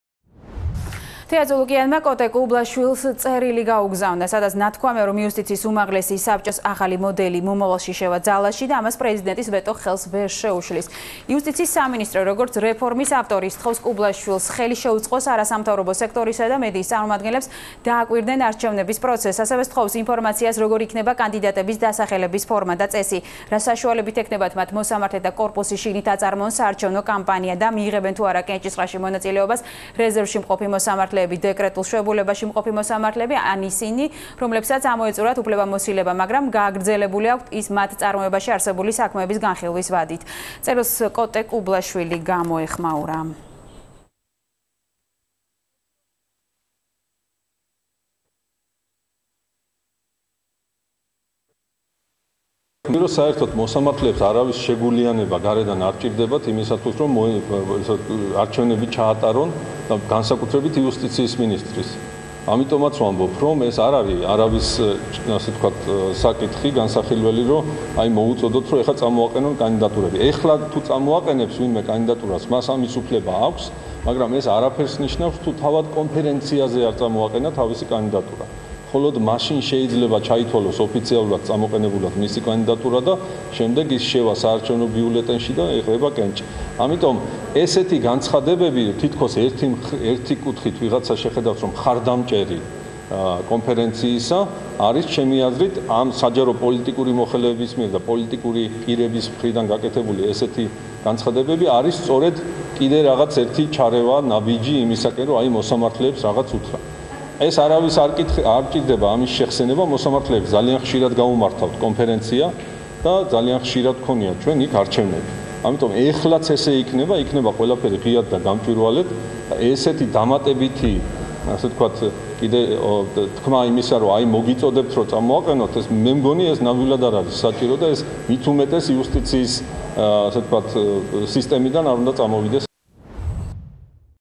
წულუკიანის წერილი კუბლაშვილს და კუბლაშვილის პასუხი
სასამართლოში გასატარებელი რეფორმების თაობაზე თეა წულუკიანი უზენაესი სასამართლოს თავმჯდომარეს წერილით მიმართავს.